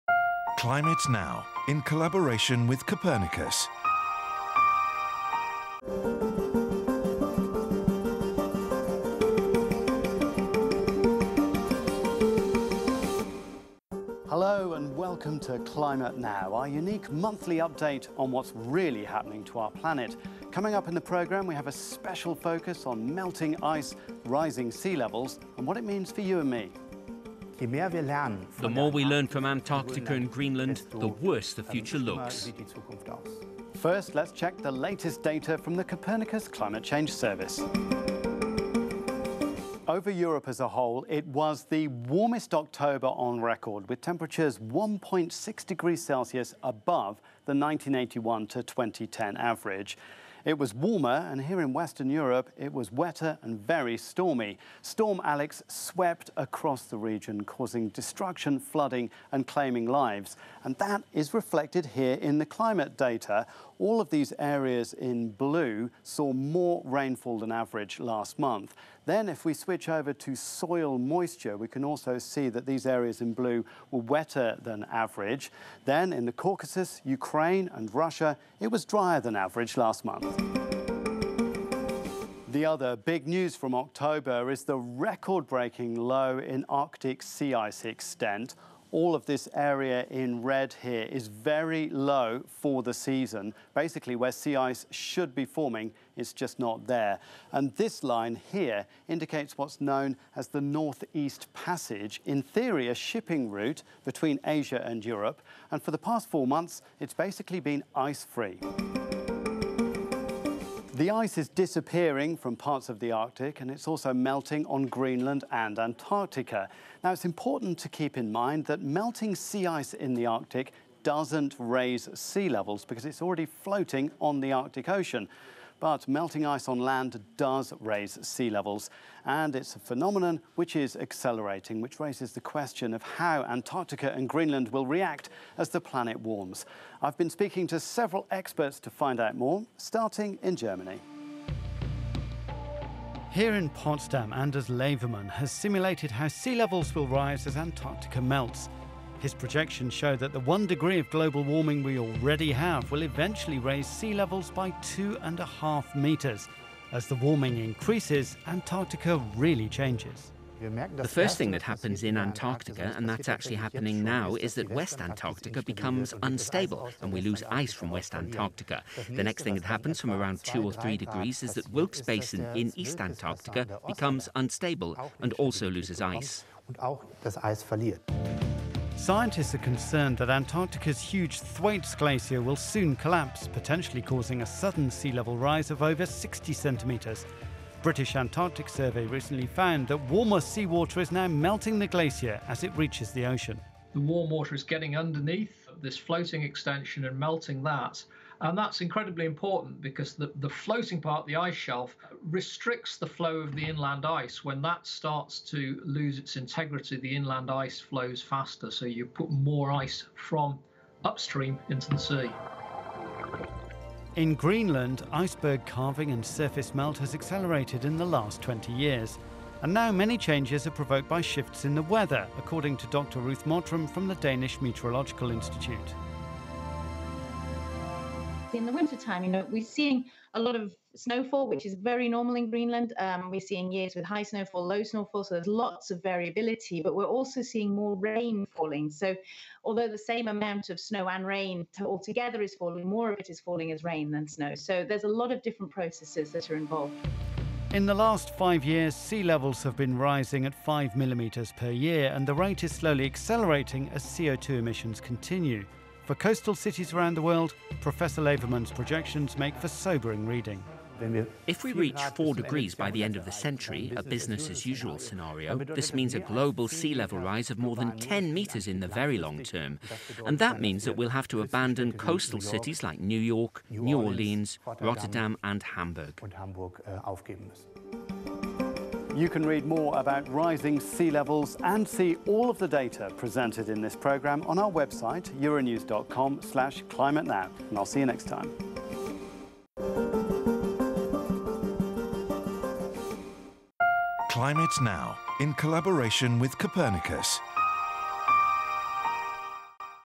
Climate Now by Copernicus - October 2020
Over Europe as a whole, it was the warmest October on record, with temperatures 1.6 degrees Celsius above the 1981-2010 average. October was also a record-breaking low in Arctic sea ice extent.

In this episode of Climate Now, we take a closer look at melting ice and rising sea levels, to understand what's really happening at the poles as the planet warms.